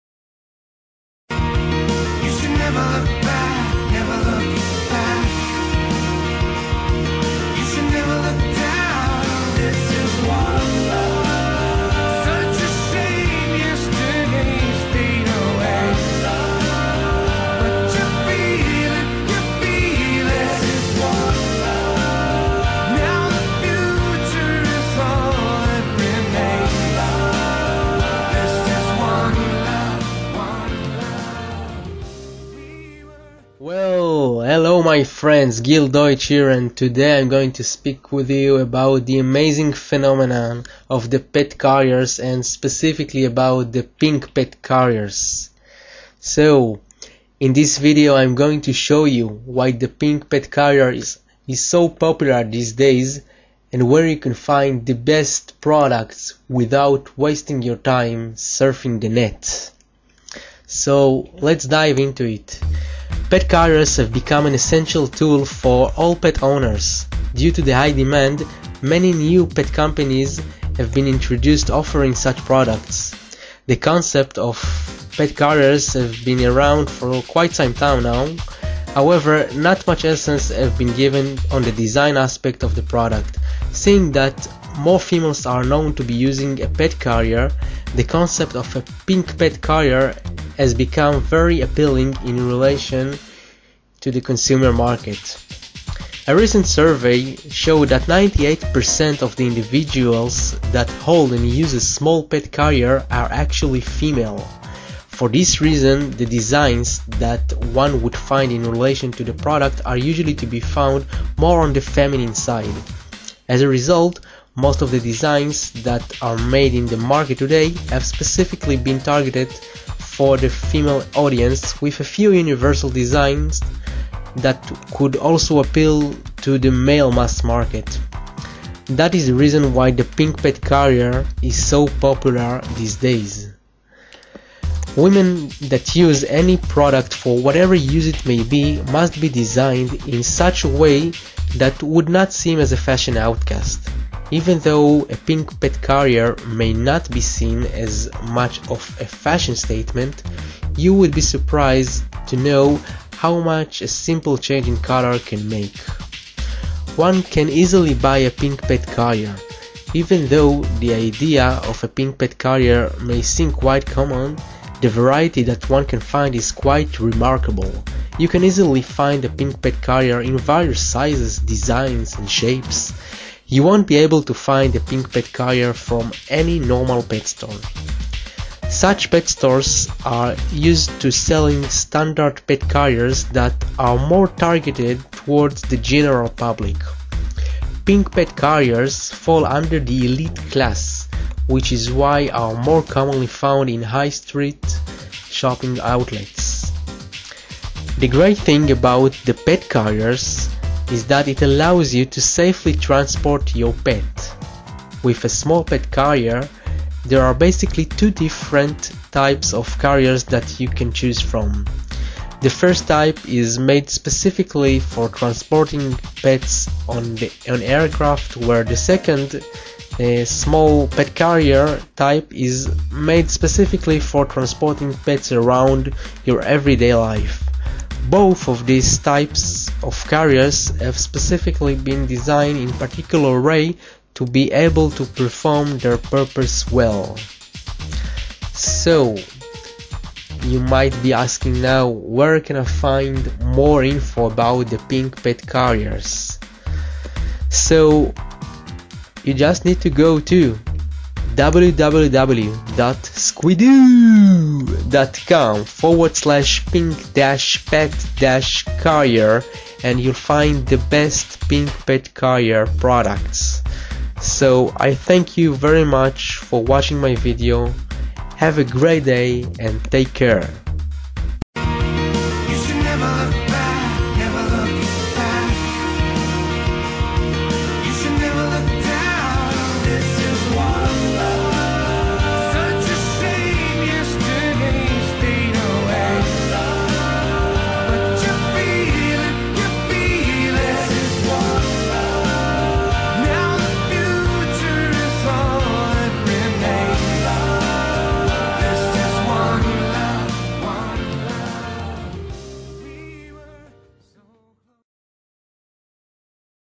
The Pink Pet Carrier Phenomenon
Are you looking for a pink pet carrier, or any other small pet carriers?
This video will show you why the Pink Pet Carrier Is so popular these days and where you can find the best products without wasting your time.